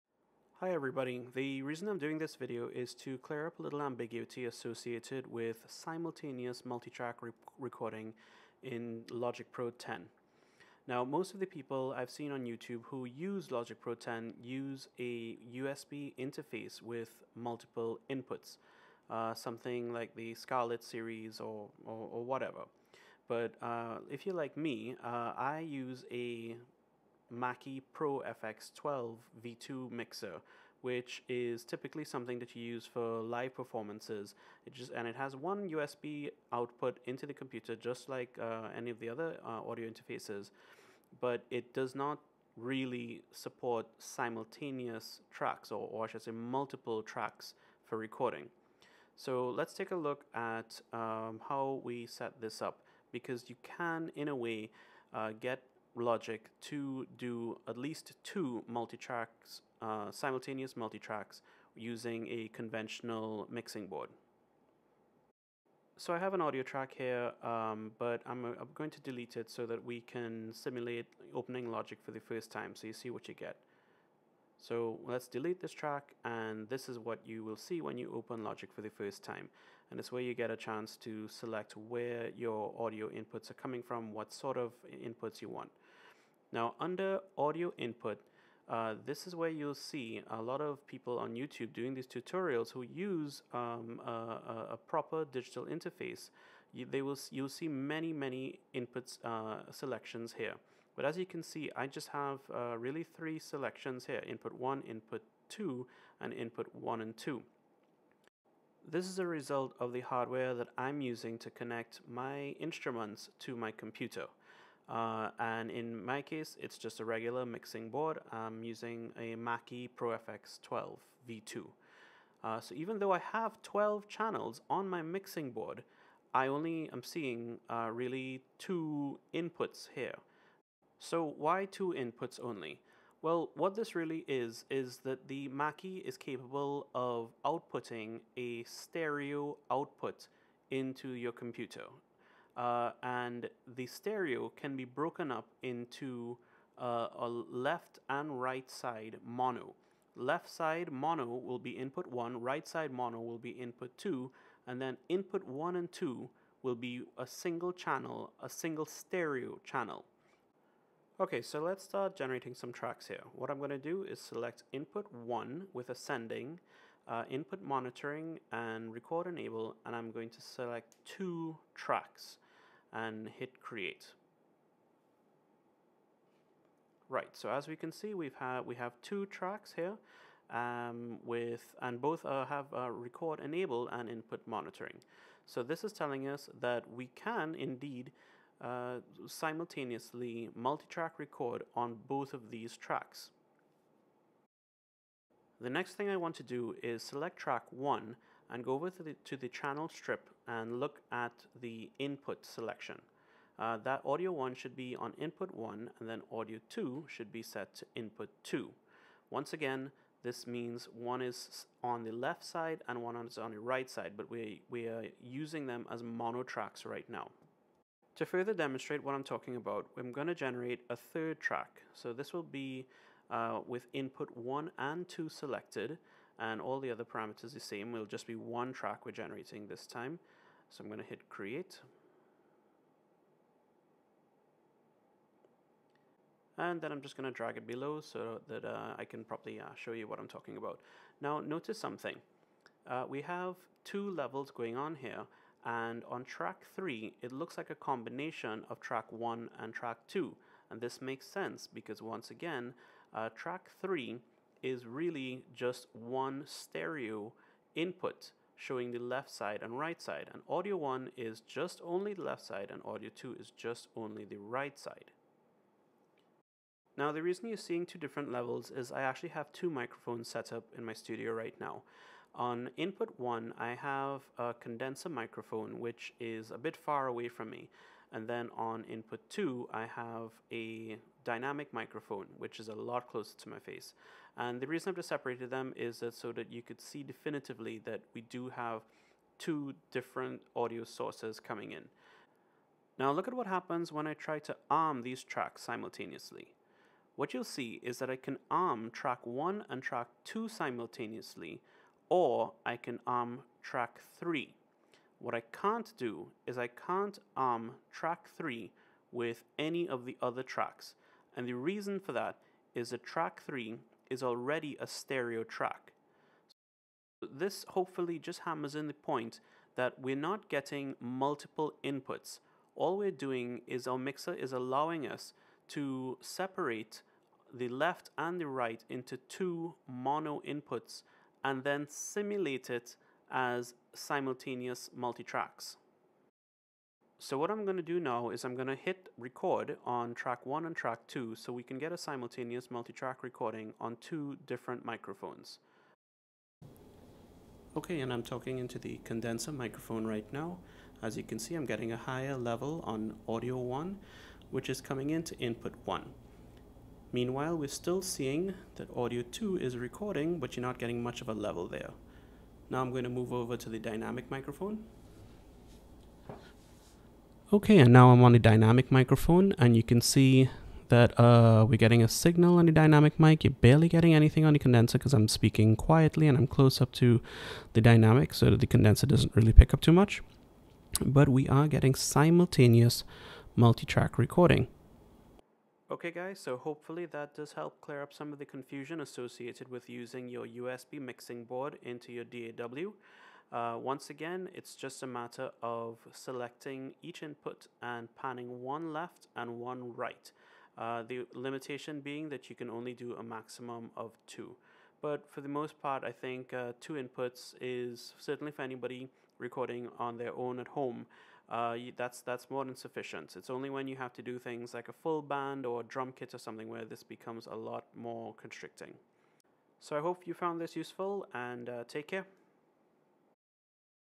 Simultaneous Multitrack Recording With Multiple Channel Mixing Board In Logic Pro X
How to accomplish Simultaneous Multitrack Recording with a USB Mackie ProFX12 V2 mixing board.